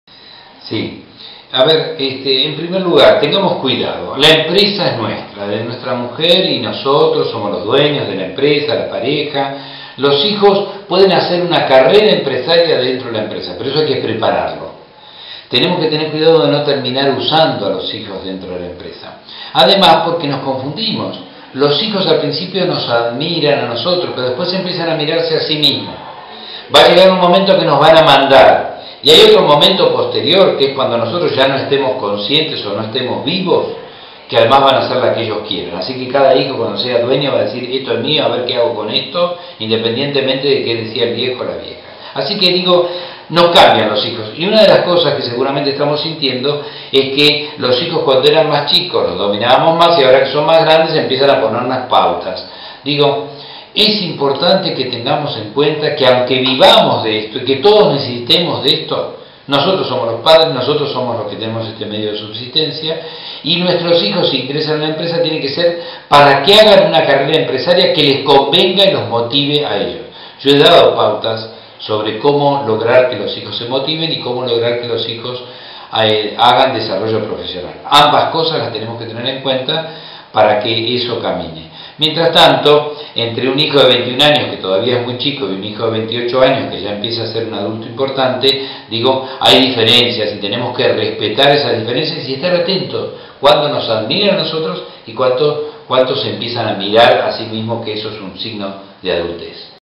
EF 6 - ¿Cómo despertarles interés junto con mi esposa a mis hijos ....
Empresa Familiar - ¿Cómo despertarles interés junto con mi esposa a mis hijos de 21, 23, 25 y 28 para potenciar y expandir la empresa? Somos 6 y siempre tuvimos el apoyo de los hijos.
El consultor José María Quirós responde las preguntas de los empresarios